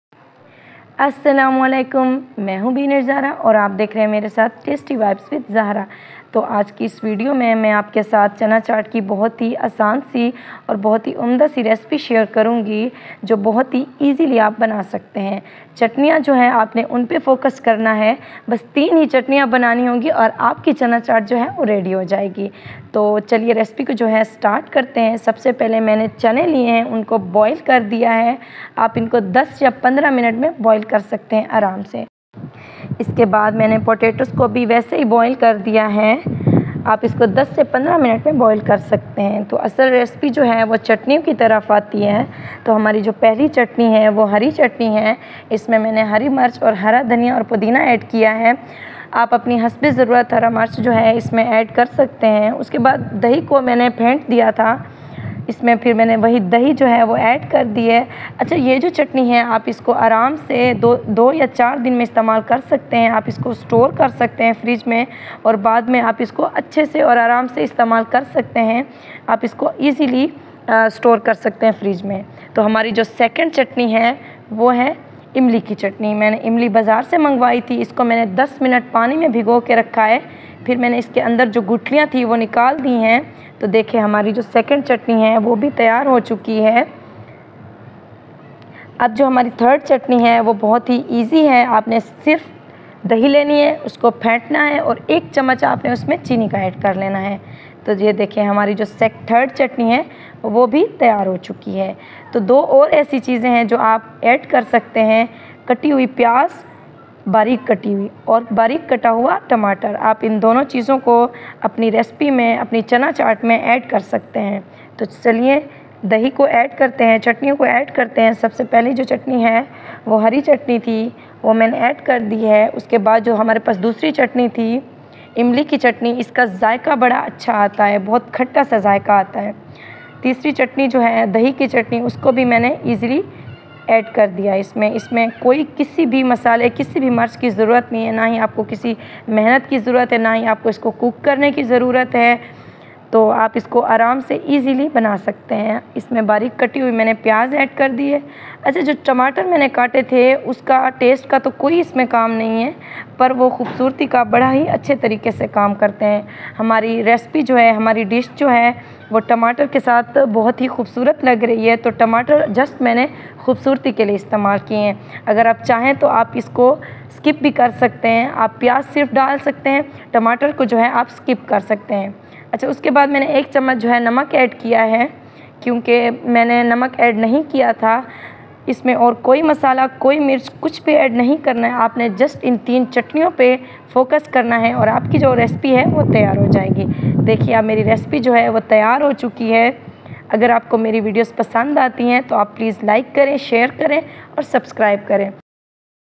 Chana Chaat By Tasty vibes With Zahra | Special Chana Chat Recipe 2023
Welcome to "Tasty Vibes With Zahra"! Get ready to tantalize your taste buds with our latest culinary delight: "Special Chana Chaat Recipe 2023." In this mouthwatering video, join us as we take you on a flavorful journey through the world of chaat.

Looking to elevate your snack game? Look no further! Our Chana Chaat recipe is a modern twist on a classic favorite, curated to perfection by Zahra. With a harmonious blend of spices, textures, and ingredients, this dish promises to be a sensational treat for your senses.

Zahra, our seasoned culinary expert, will guide you through the step-by-step process, making it easy for beginners and experienced chefs alike to recreate this dish at home. From cooking the chickpeas to preparing the tangy tamarind chutney and selecting the freshest garnishes, you'll learn the art behind crafting a truly remarkable Chana Chaat.

Whether you're hosting a party, craving a delightful snack, or simply exploring new flavors, this Special Chana Chaat Recipe for 2023 is the answer. The fusion of traditional elements with contemporary flair will leave you craving for more.

Don't miss out on this delectable recipe that promises an explosion of flavors in every bite. Join us in the kitchen to create a taste of 2023 that you won't forget!